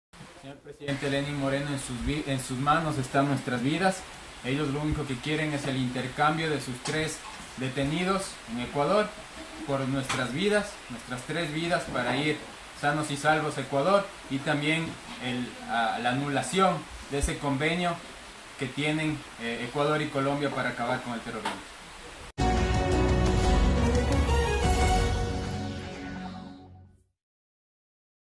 Fe de vida del equipo periodístico ecuatoriano del diario El Comercio
Video cortesía de NTN24.